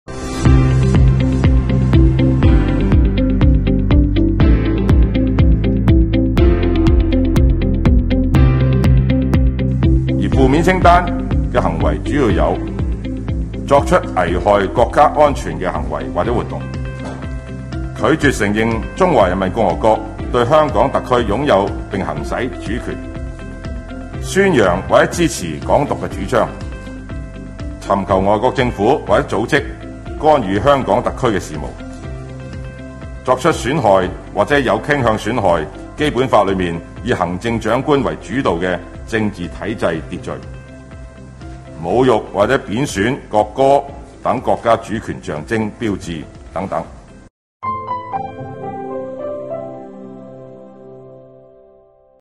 香港拟修订公职条例 曾国卫明确公职人员“负面清单”
2月23日，香港特区政府公布公职条例草案的修订内容，以完善公职人员宣誓安排，进一步确立“爱国者治港”的基本原则。香港特区政府政制及内地事务局当天下午举行记者会，讲解《2021年公职（参选及任职）（杂项修订）条例草案》。